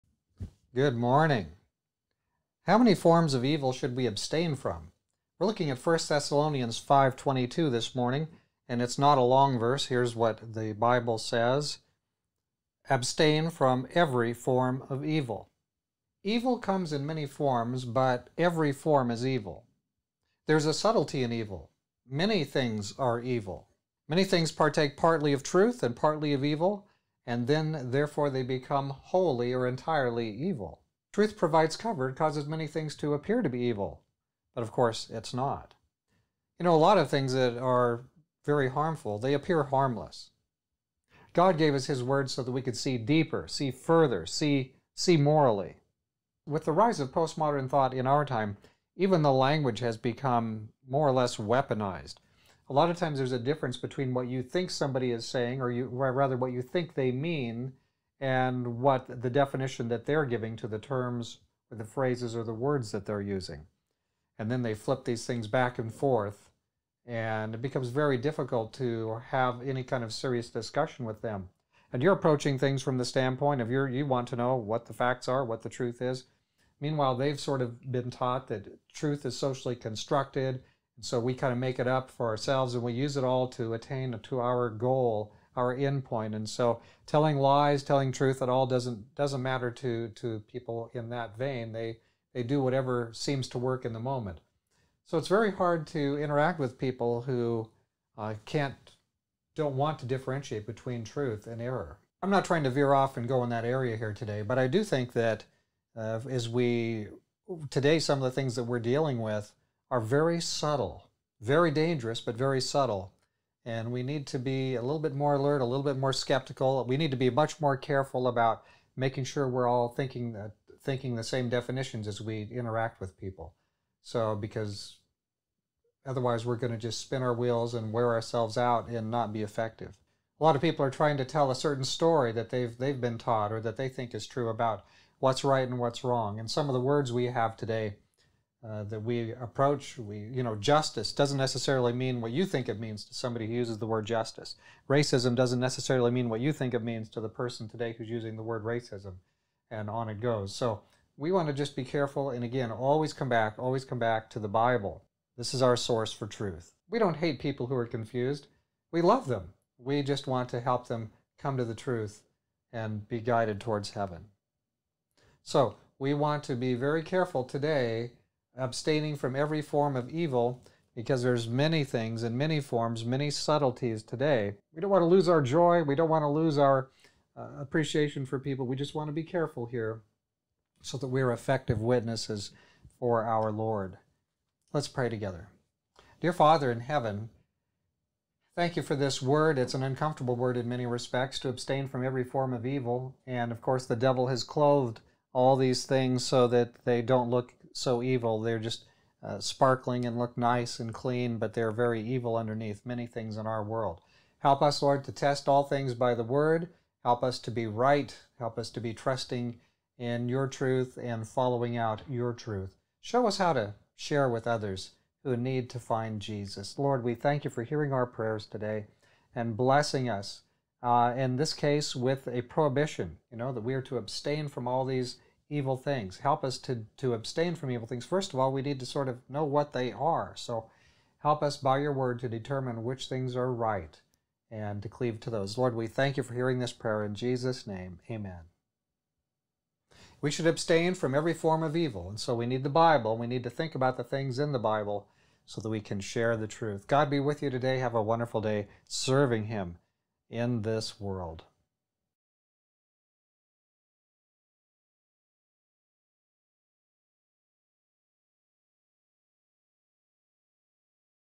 Jesus is Coming 1 Thessalonians 5:22 Every Form of Evil
How many forms of evil should we abstain from?

Parler.com: @SecondComing
Twitter.com: @SaviorAndMyKing